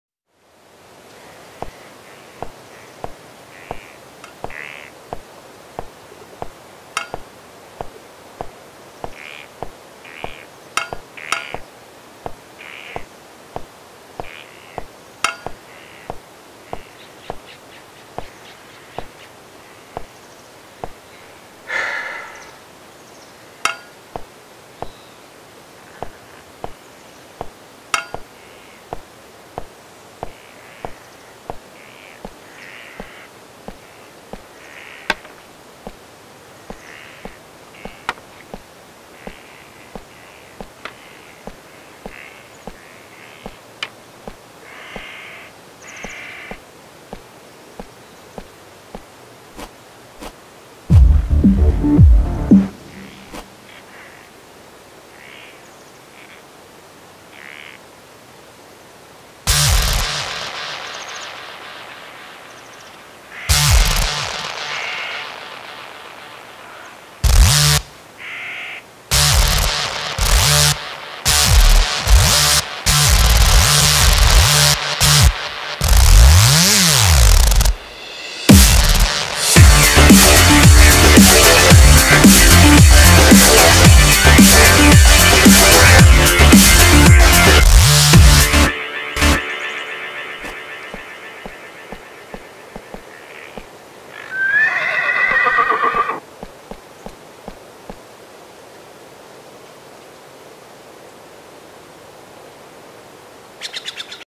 Electric Fence
The overall purpose of this project was to learn how to use Sound Effects effectively in order to enhance your film, and I think we did that quite well.
It was awesome working with DJ Serenity and it was definitely a new experience having music that was custom made for our video.

Definitely check DJ Serenity out at his and check out his many original tracks and remixes!